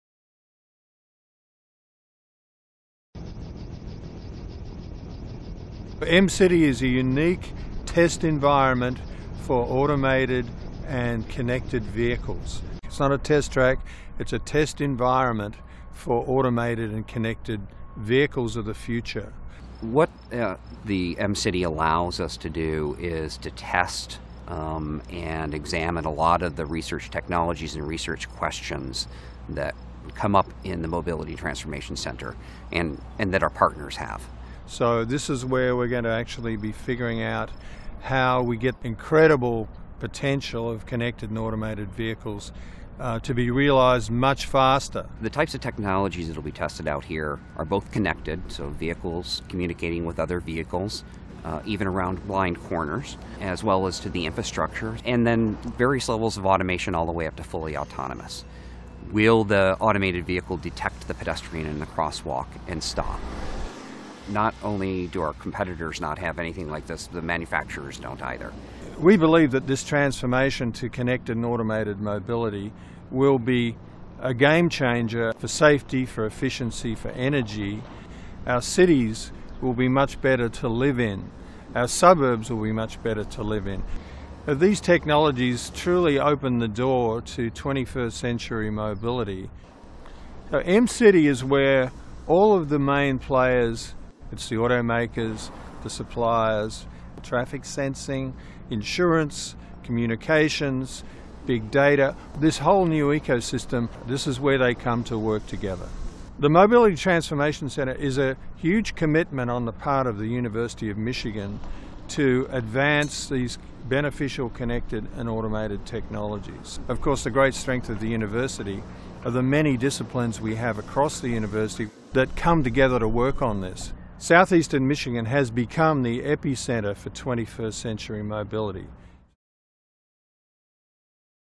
Mcity Overview
FOR AN UPDATED VERSION OF THIS VIDEO.
An overview of the Mcity connected and automated vehicle test facility at the University of Michigan's Mobility Transformation Center.